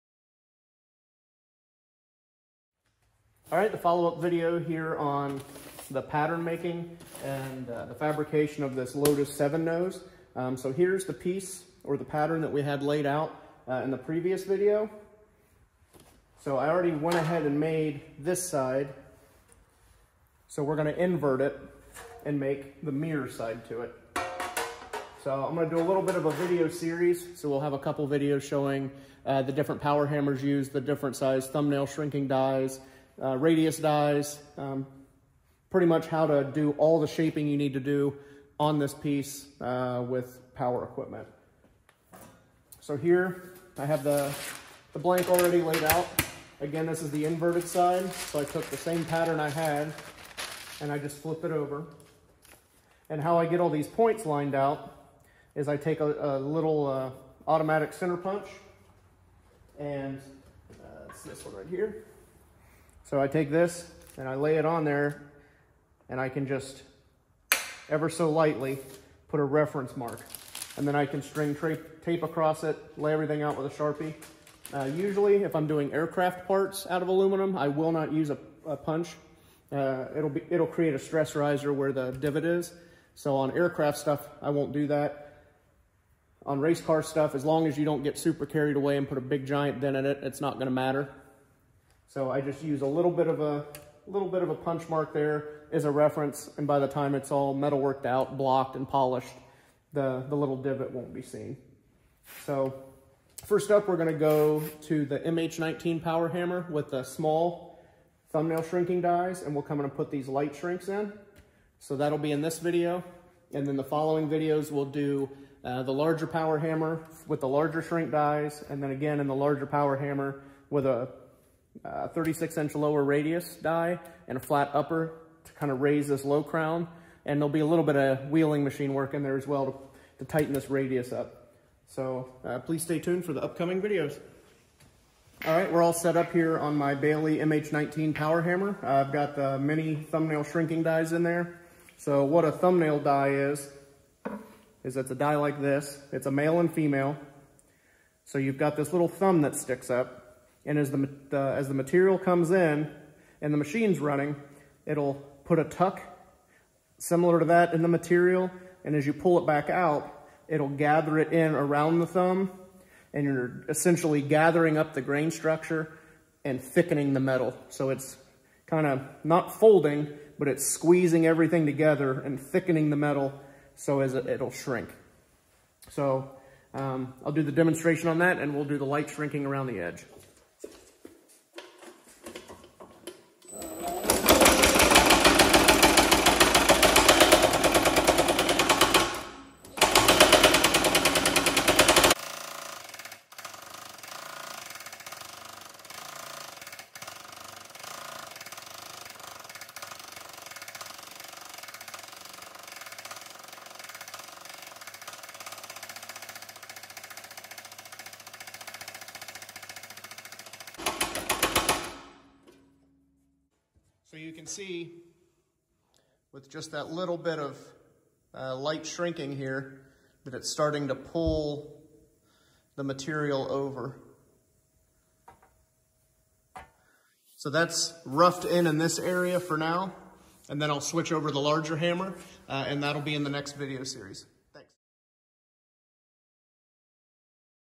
Shaping a Lotus 7 Nose - part 1
In part 1 of this short video series on making the Lotus 7 nose I show how to do a basic pattern lay out ( pattern video is on here), then perform initial shrinking operations on the power hammer using thumbnail shrinking dies.